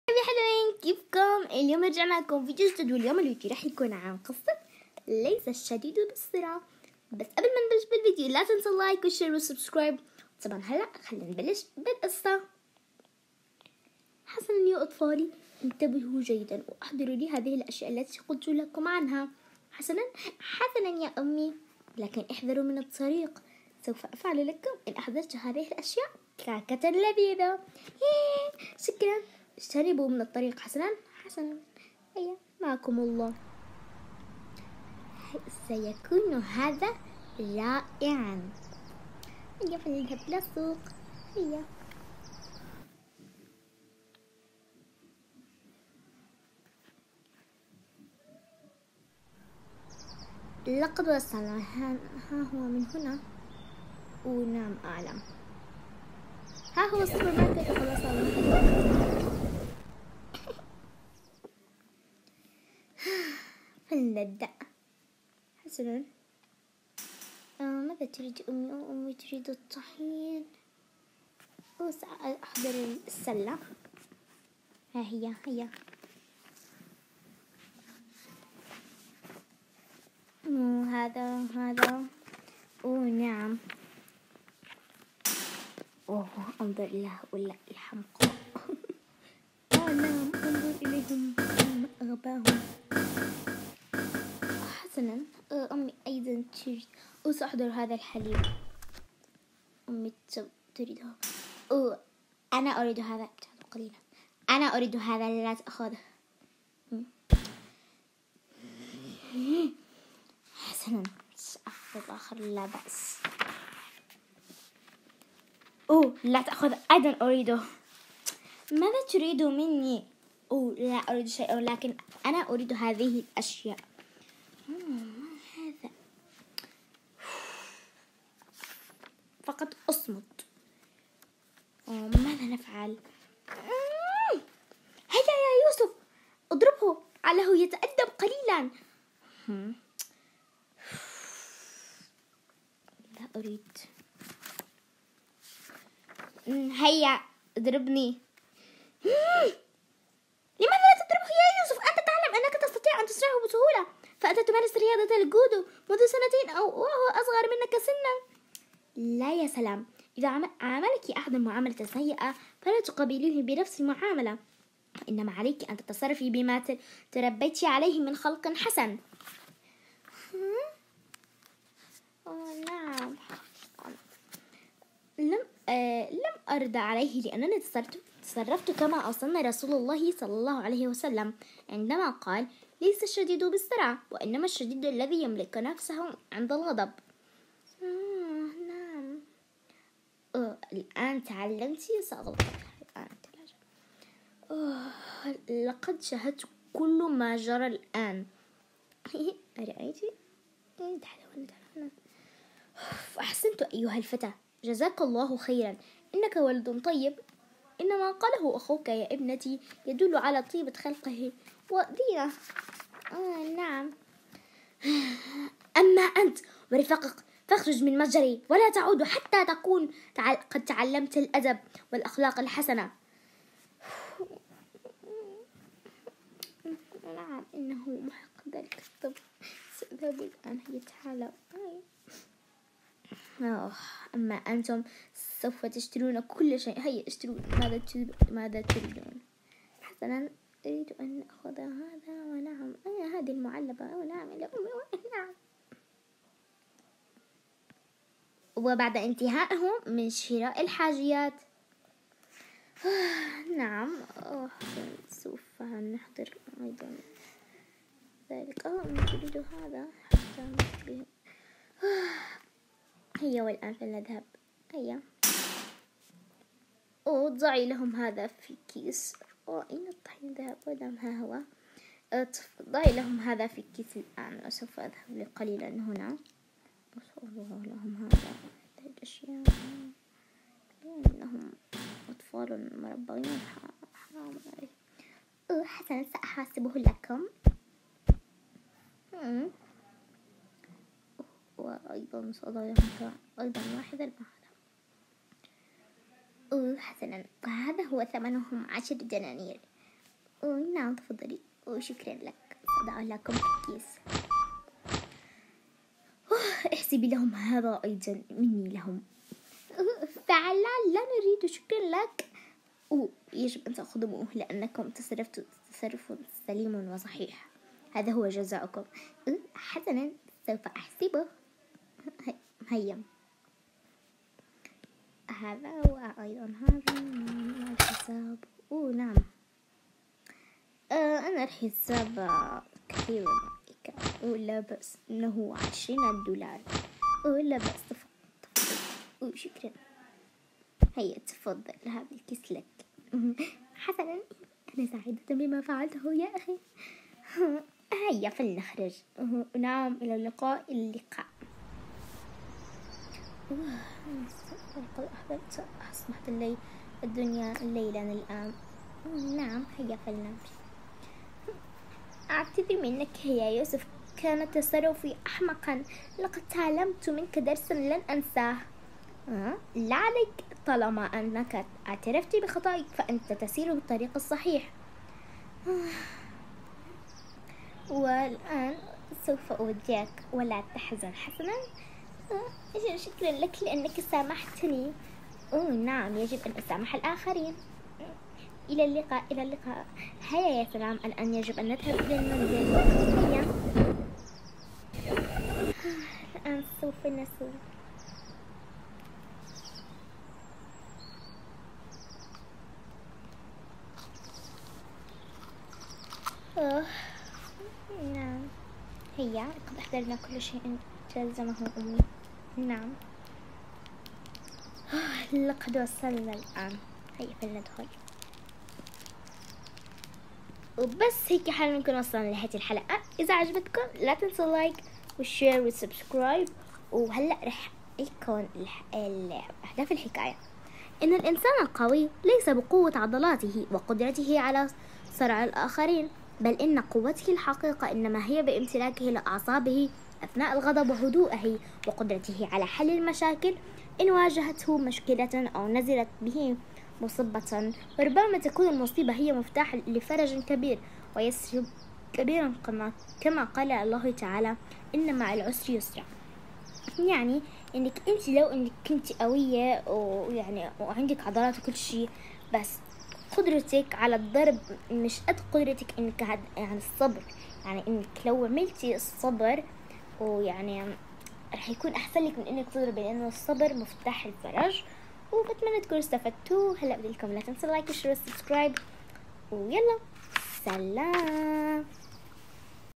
قصة ليس الشديد بالصرعة🧒🏻👱🏻‍♀️🧖🏼‍♀️🐵
القصة تتكام
ام طلبت من ابناها الاثنين الذهاب الي المتجر لشراء حاجيات عمل كعكة فذهبا الي المتجر فراى 3 اطفال يريدون القتال معهم لكن ابن الام لم يرد القتال معهم وقال لهم عن حديث الرسول صلى الله عليه وسلم وفرح العامل بتصرف الطفل اعطاهم مكافئة